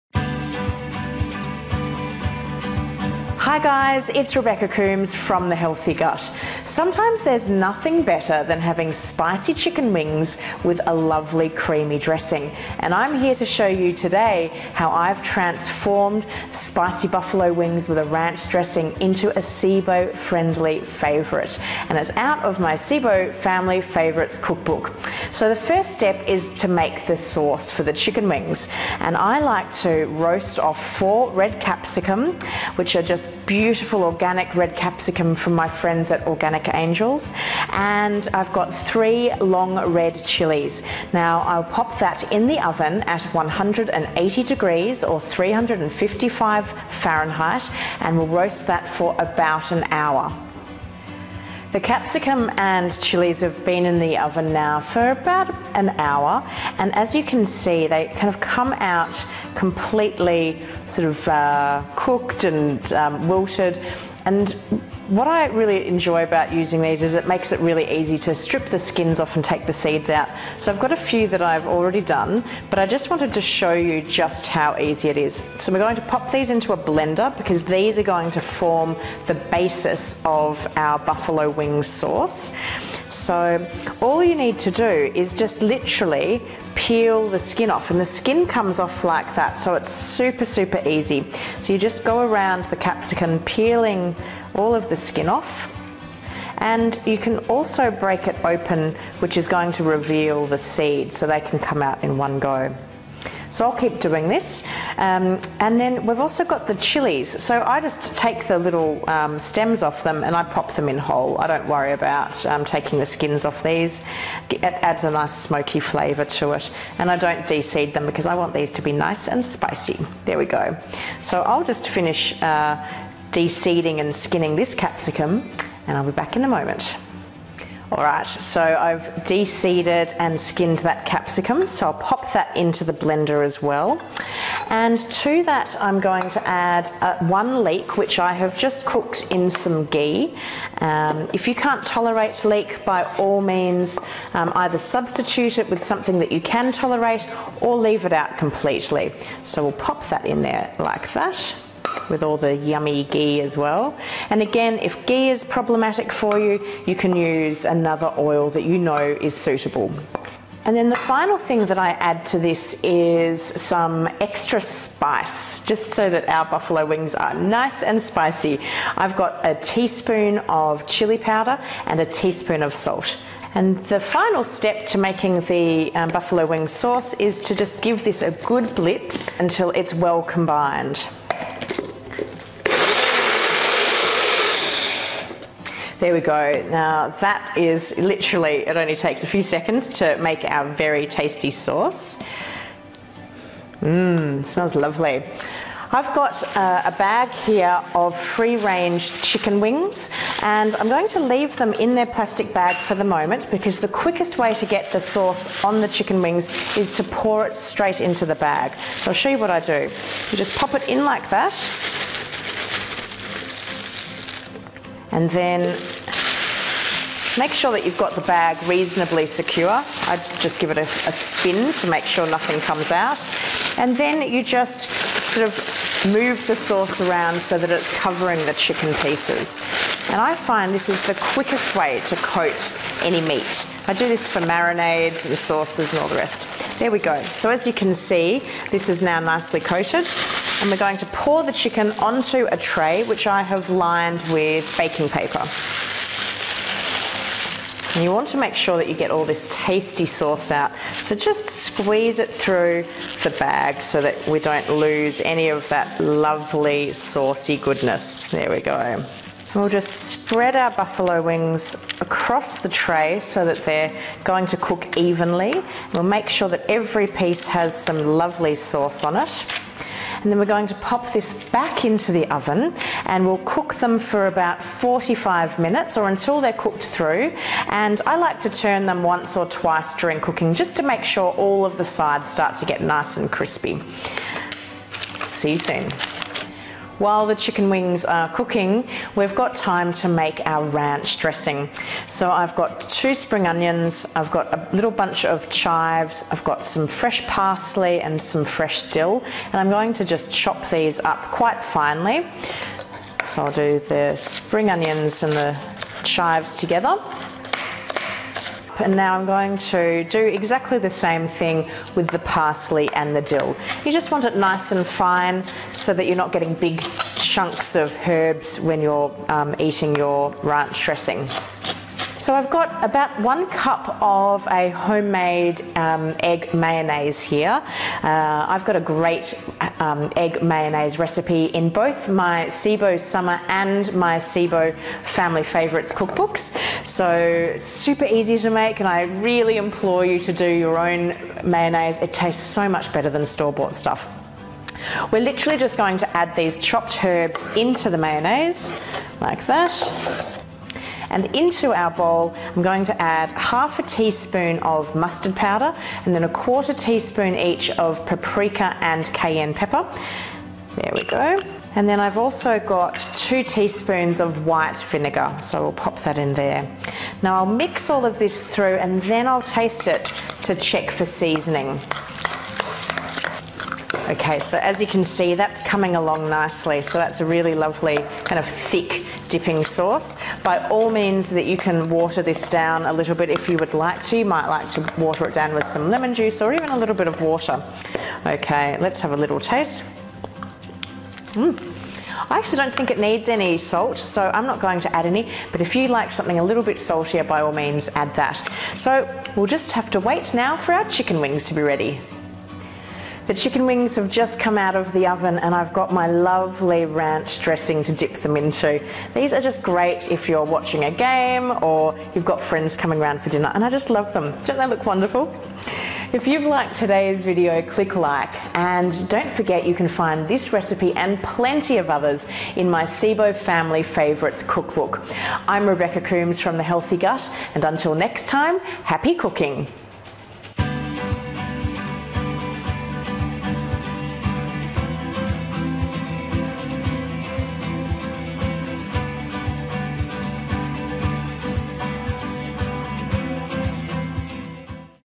Spicy Buffalo Wings | Rebecca Coomes, The Healthy Gut
The perfect game-day snack is here. These spicy buffalo wings, topped with a cool ranch dressing, are sure to be a winner. With these SIBO-friendly wings, you don't have to feel like you're missing out on all the game-day fun.

This recipe is suitable for Phase 1, Restricted of the SIBO Bi-Phasic Diet Protocol.  It is also:

-Gluten free
-Dairy free
-Grain free
- Low carbohydrate
- Low FODMAP